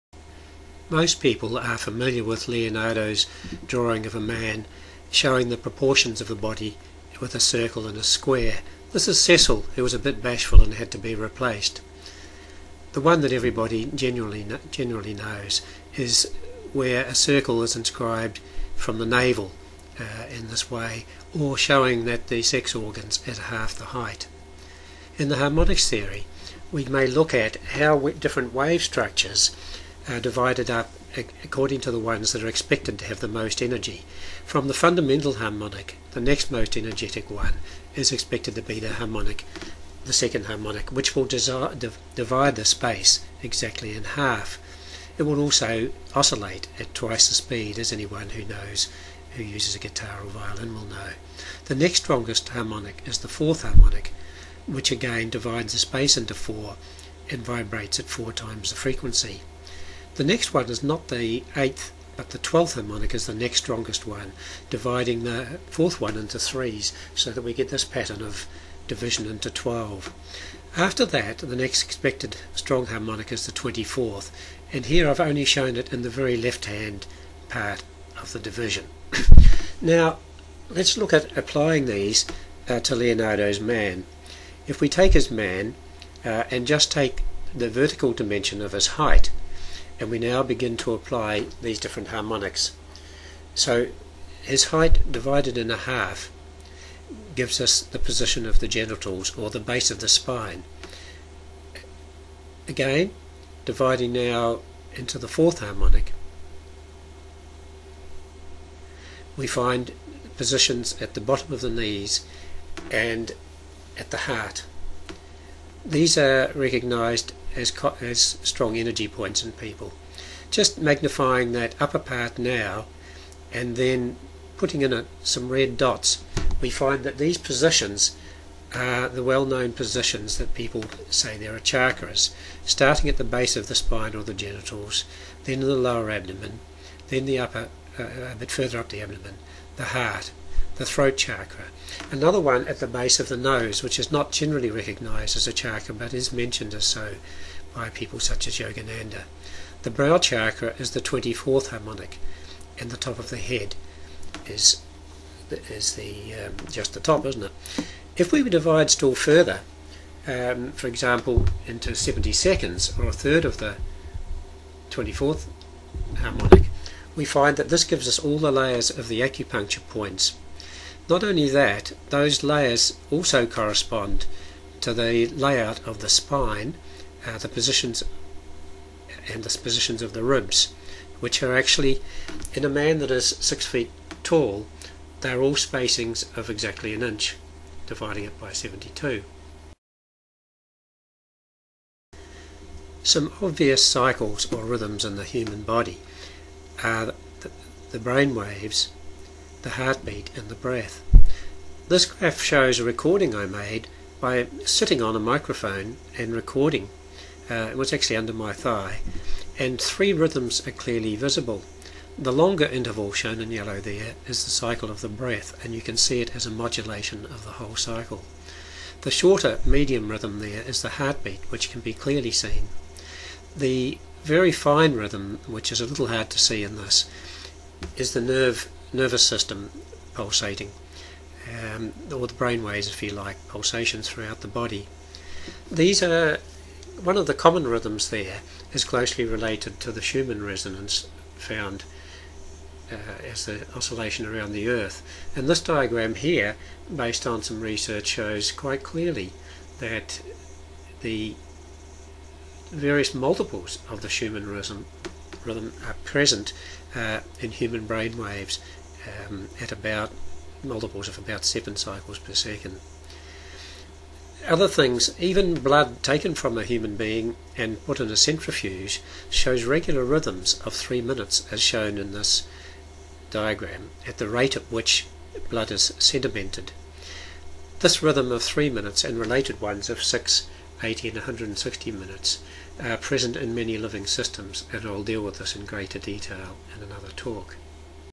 Human Harmonics - Chakras & Acupuncture points
When the Harmonics Theory is applied to the vertical dimension of a human being it predicts the locations of the chakras, or strong energy points, as well as acupuncture layers. The subtle energies of the human body and the location of the nodes of the nervous system are the result of standing waves of the nervous system.

The acupuncture diagrams are by Carole and Cameron Rogers. I mistakenly stated that the  brain waves that were multiples of the Schumann resonance were human, but this diagram was actually of a rat's brain wave frequencies. Humans also show Schumann related frequencies.